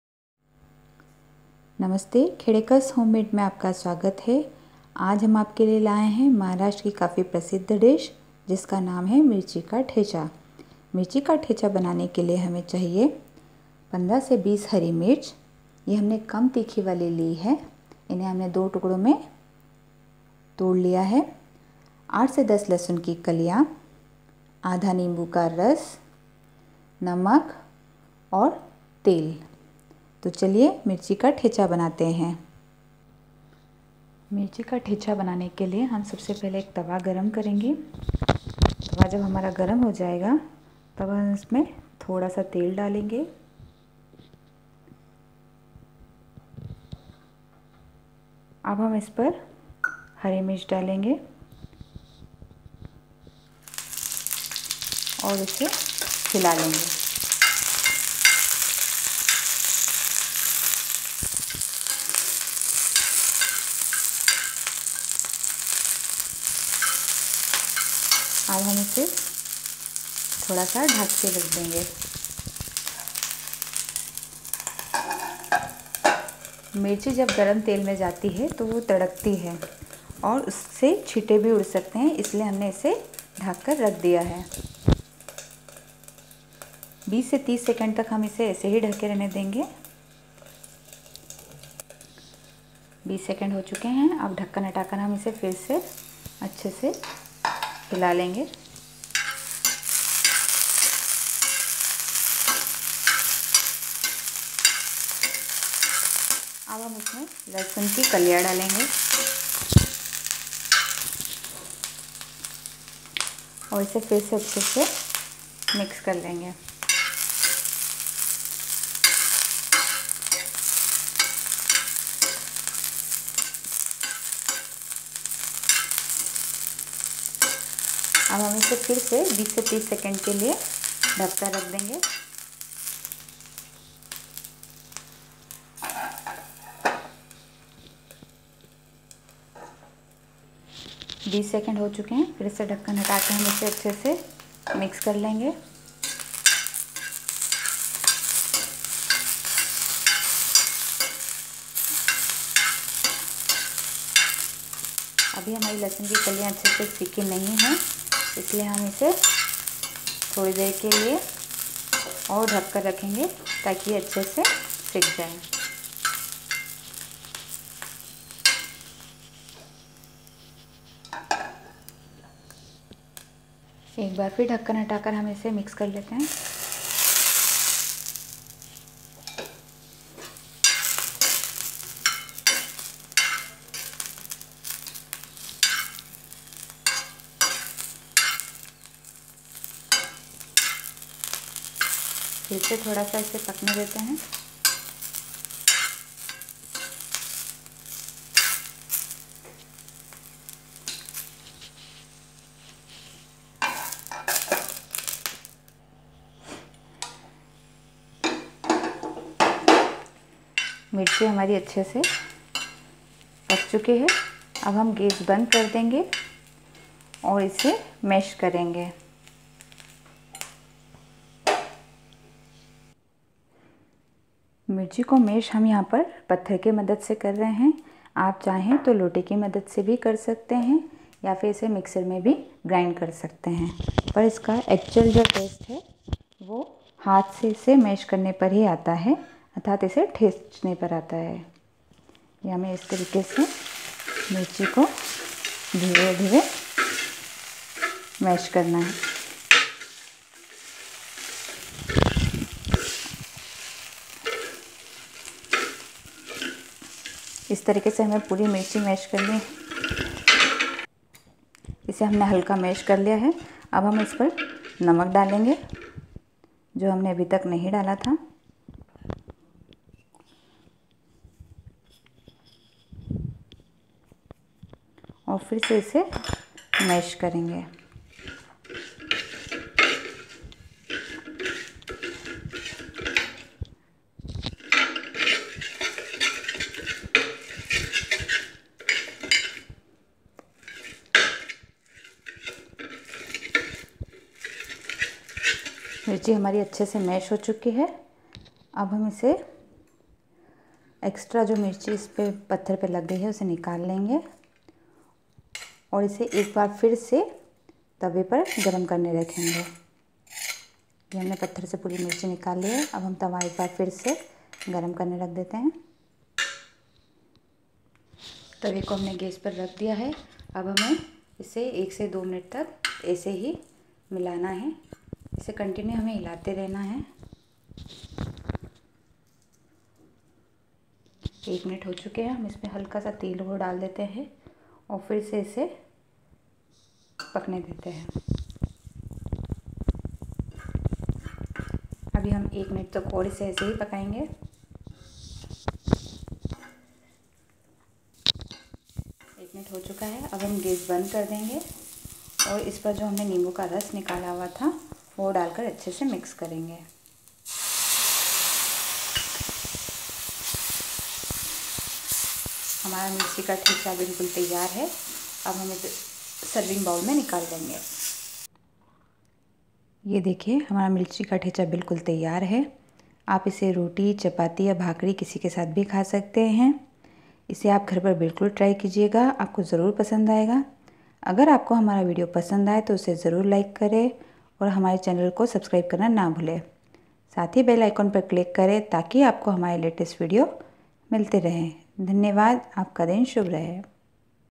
हिरवी मिरची चा ठेचा / मिर्ची ठेचा / Mirchi Ka Thecha
सामग्री हिरवी मिरची ठेचा / Ingredients Mirchi Ka Thecha:-
15-20 हरी मिर्ची - 15-20 Green Chili
8-10 लहसून की कली - 8-10 Garlic Clove
तेल - Oil
नमक - Common Salt
नींबू का रस - Lemon Juice